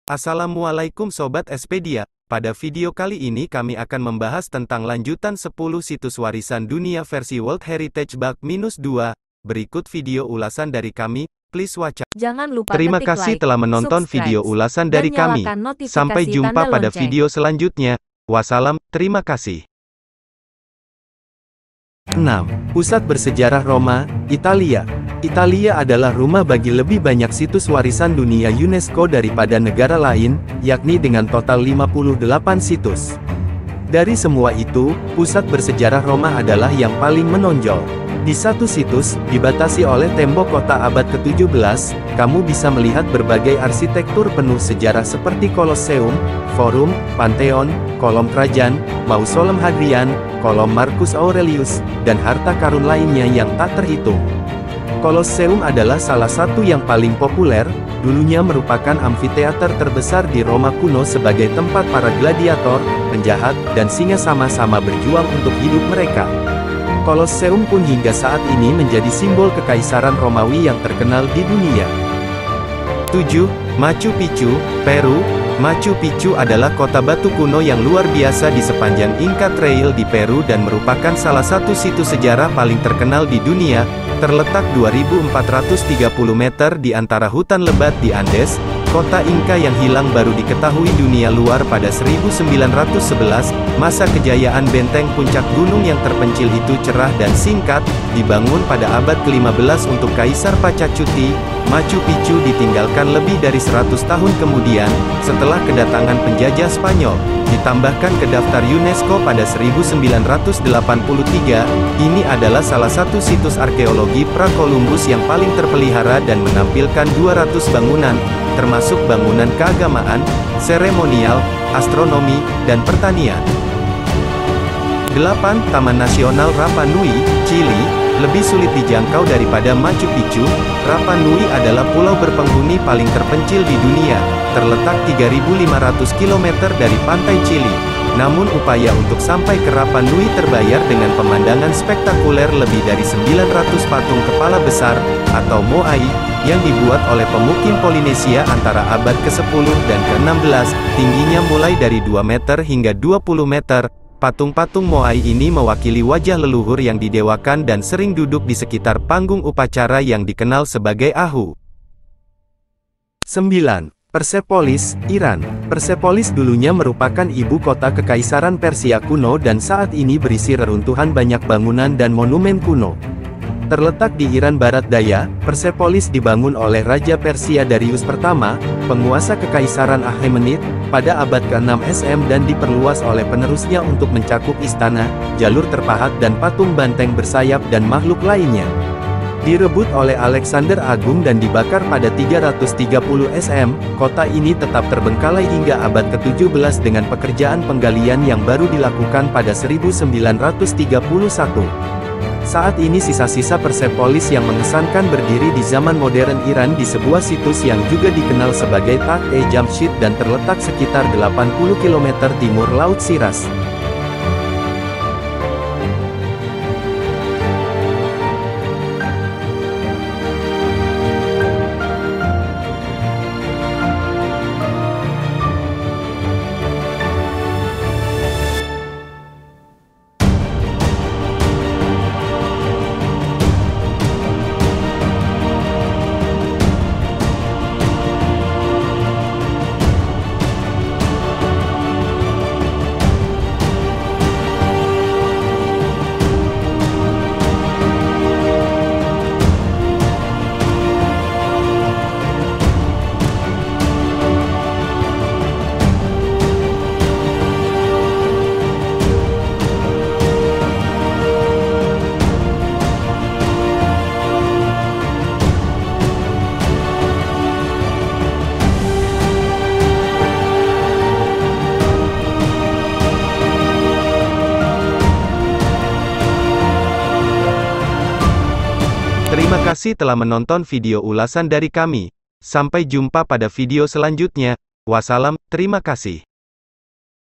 AMAZING !!! Daftar 10 Situs Warisan Dunia UNESCO Terpopuler di Berbagai Belahan Dunia Bag-2
AMAZING !!! Daftar 10 Situs Warisan Dunia UNESCO Terpopuler di Berbagai Belahan Dunia Bag-2 || @espediainfo99
Ada banyak situs warisan dunia yang diakui oleh United Nations Educational, Scientific and Cultural Organization (UNESCO).
Situs warisan dunia pun dibuat oleh UNESCO sebagai area atau landmark yang dipilih oleh UNESCO sendiri karena kepentingan ilmiah, sejarah, atau budayanya.
Hingga saat ini, terdapat 1.154 situs dalam World Heritage List atau Daftar Warisan Dunia yang dilindungi oleh UNESCO. Situs-situs termasuk beberapa tempat bersejarah terpenting di planet ini, mulai dari batu prasejarah hingga reruntuhan Romawi dan karya arsitektur abad ke-20.
Namun, mana yang harus dilihat oleh pencinta sejarah? Untuk membantu mempersempit pilihan kamu, berikut adalah 10 lokasi UNESCO paling populer di seluruh dunia, dilansir dari History Hit, Selasa (7/2/2023):